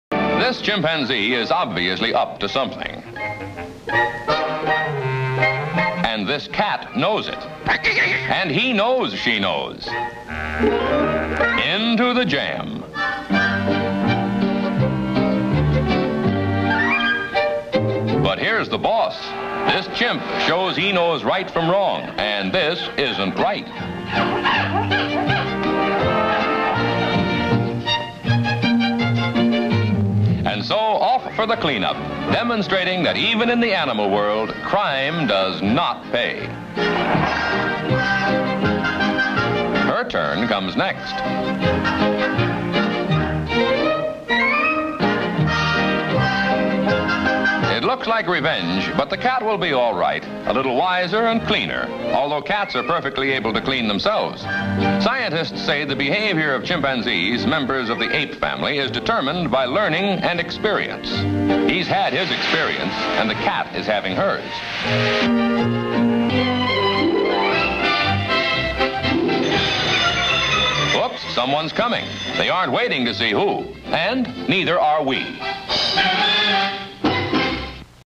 Cute chimp clip
Old footage of a cat and chimp having some adorable shinanagins